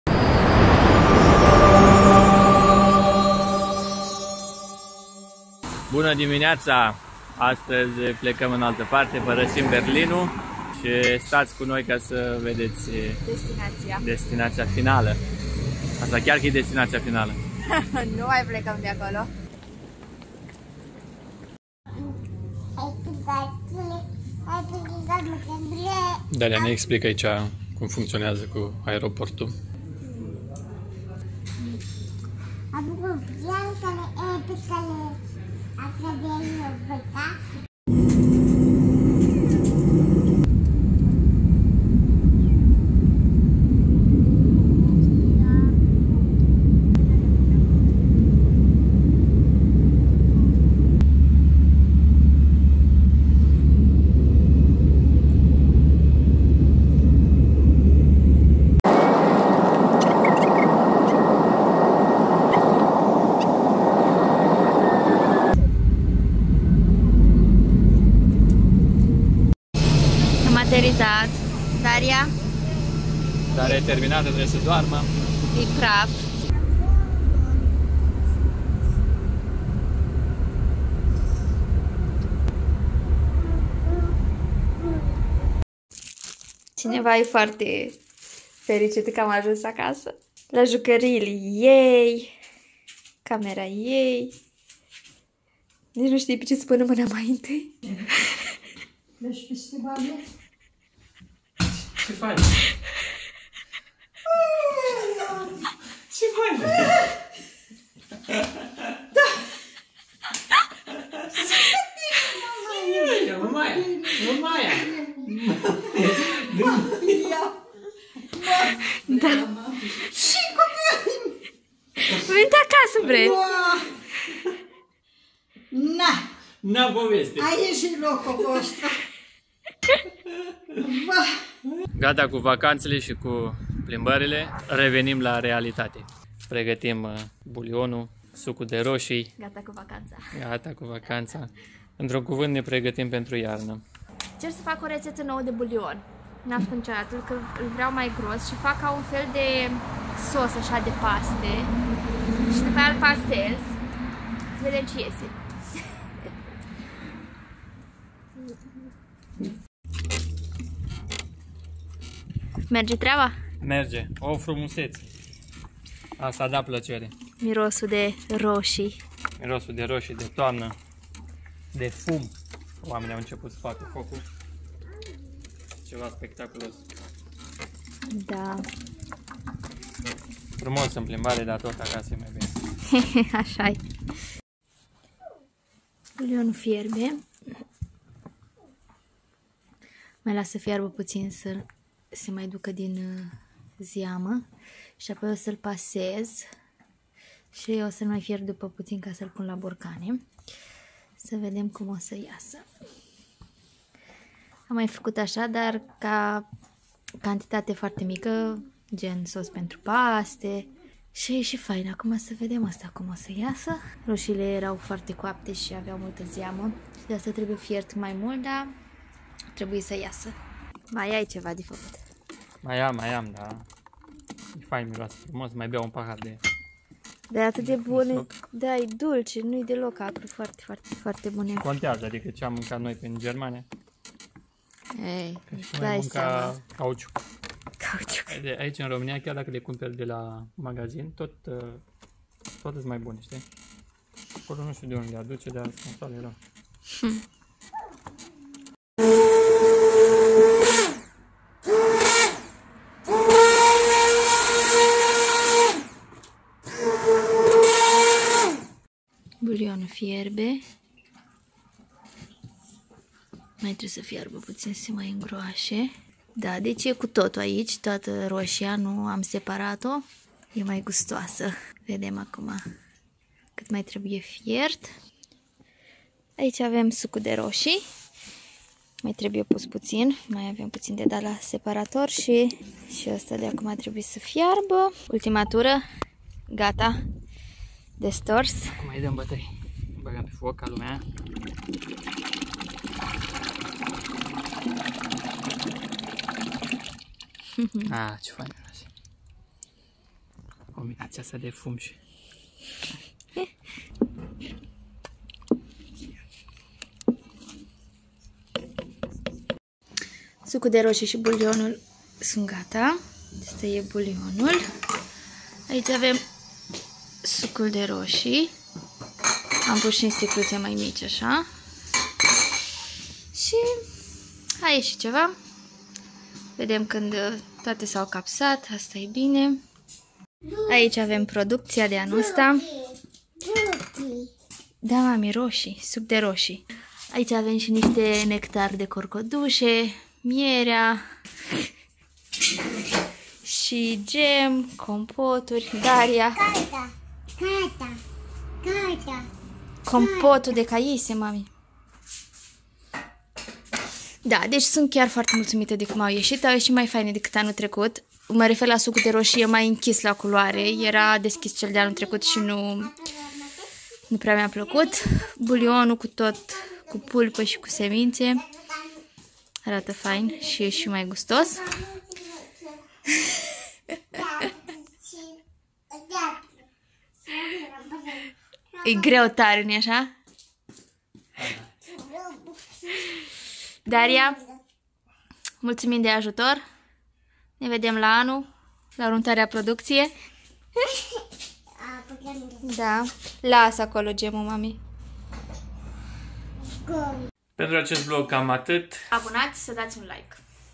Din nou acasa/ Facem suc de rosii
Ne-am intors acasa. Calatoria noastra in Germania s-a terminat mai repede decat ne-am asteptat. A fost o experienta interesanta din care am invatat multe, dar tot acasa e mai bine. Pentru ca se apropie iarna si am ajuns acasa ne-am pus pe treaba si am facut bulion si suc de rosii. Procesul il gasiti in vlog si cat de bucuroasa a fost Daria ca a ajuns acasa.

Daca v-a placut acest vlog nu uitati sa va abonati , sa dati un like si un share pentru a ne sustine.